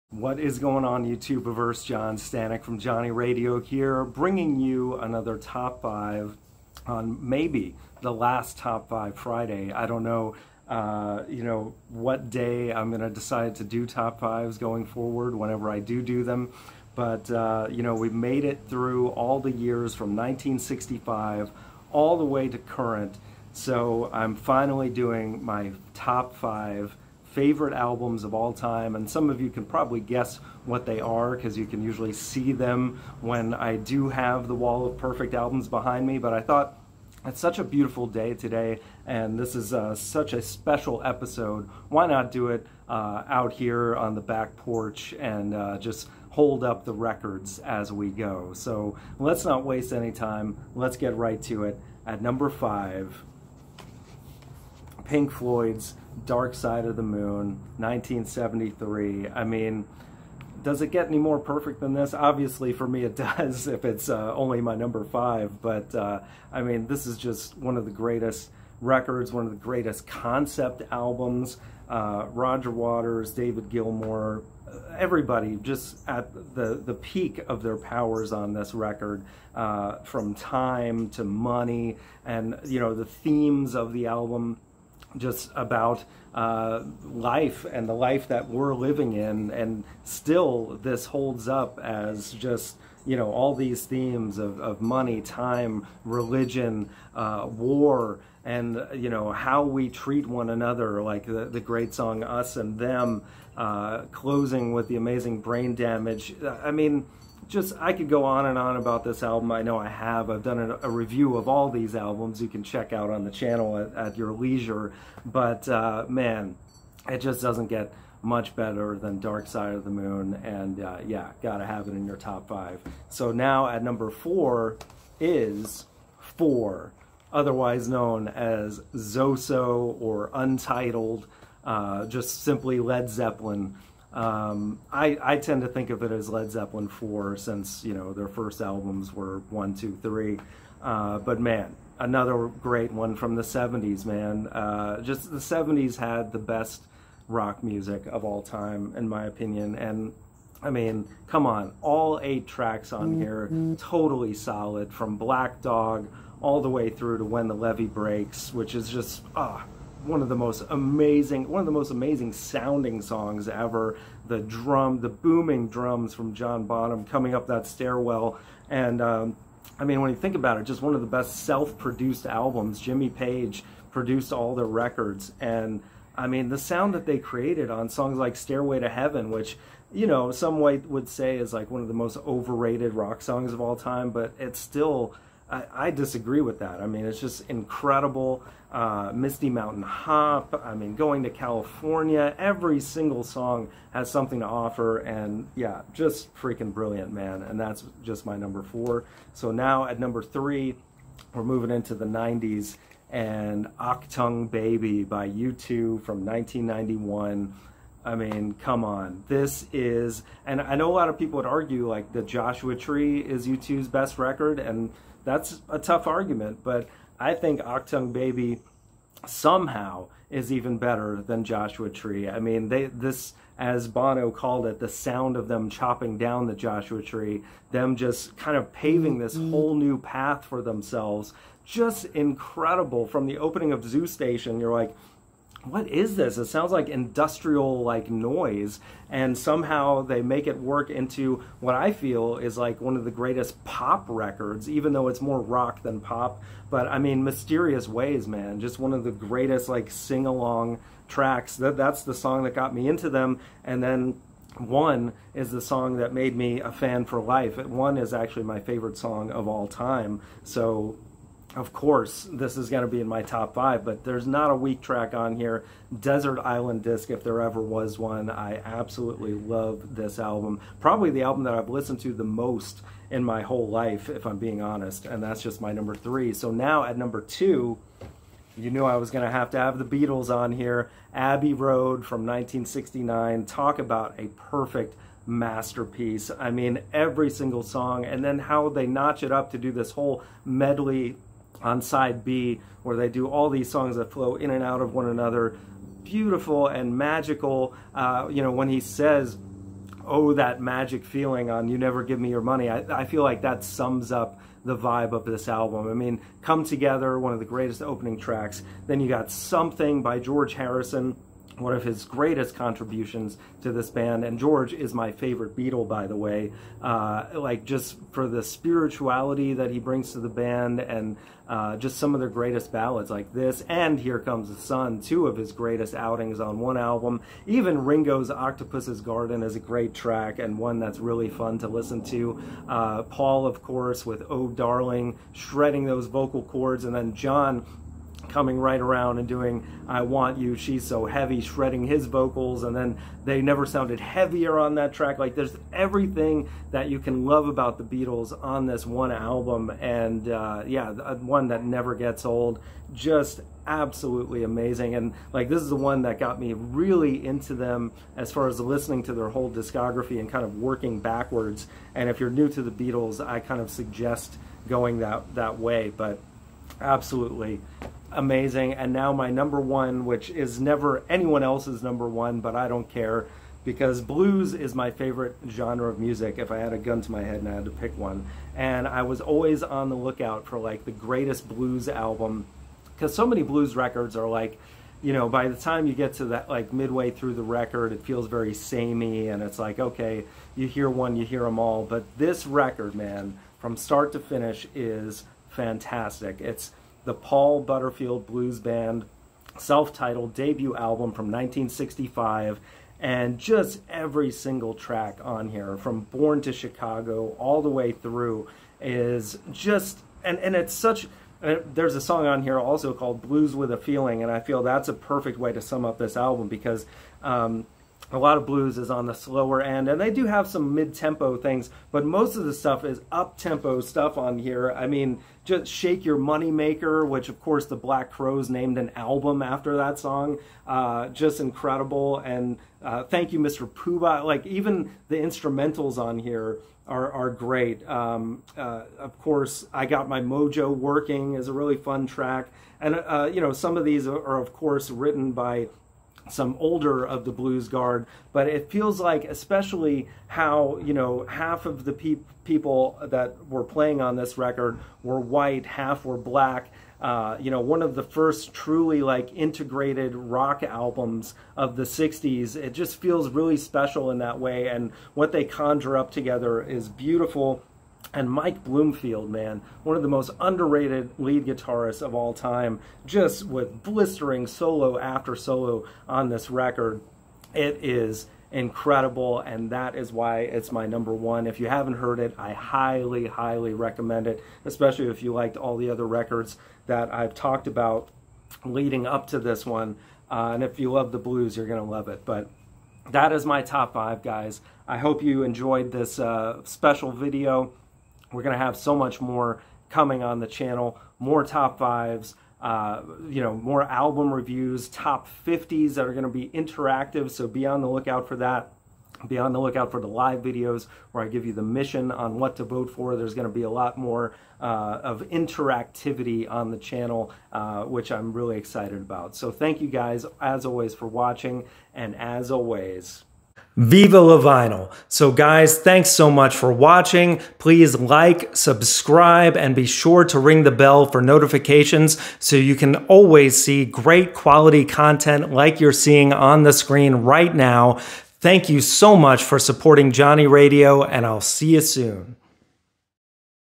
THE BEST ALBUMS OF ALL TIME
John counts down his Top 5 favorite albums of all time! What makes your list?
0:00 Intro
0:53 5
2:04 4
3:20 3
4:37 2
6:09 1
THE BEST ALBUMS OF ALL TIME, dig?